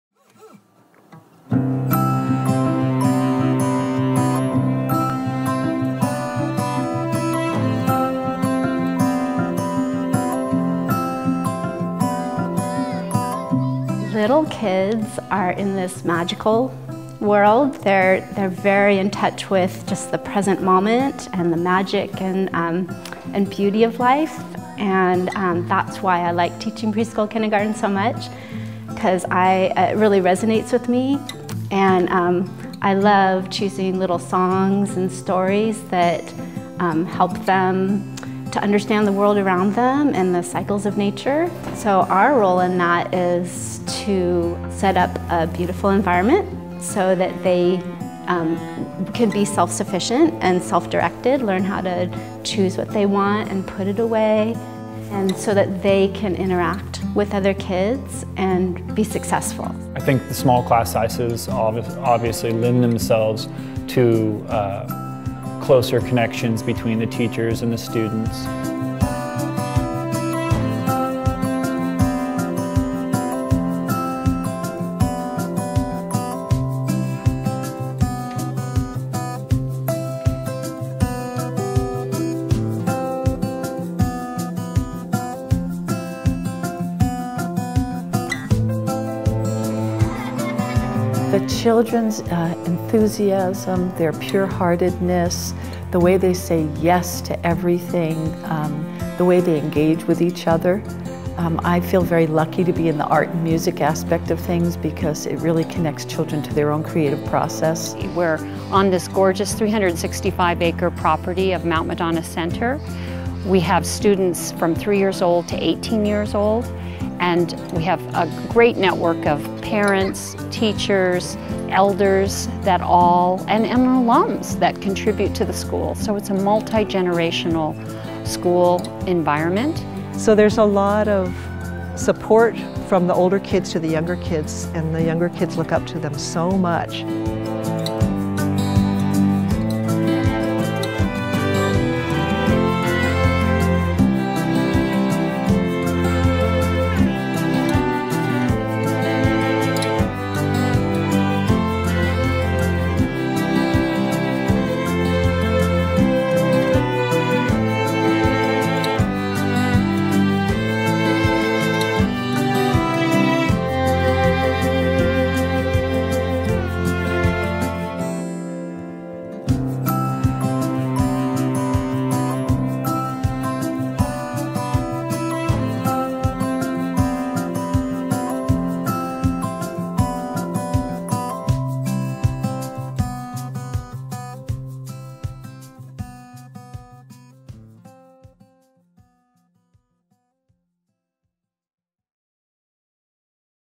The Treasure of Childhood Mount Madonna Preschool & Kindergarten
Mount Madonna School's Montessori-influenced program provides a loving, safe and stimulating learning environment where children, three to six years old, can develop at their own pace. We strive to nurture a child's innate sense of trust, goodness and wonder. At MMS, we focus on developing kindness and respect towards ourselves and others. We offer a developmentally appropriate indoor environment fostering curiosity, confidence and creativity in all aspects of the learning process. This, combined with our 355-acre outdoor classroom, empowers children to fully engage in their education. By including time for imaginative free play, children are inspired in all areas of their development.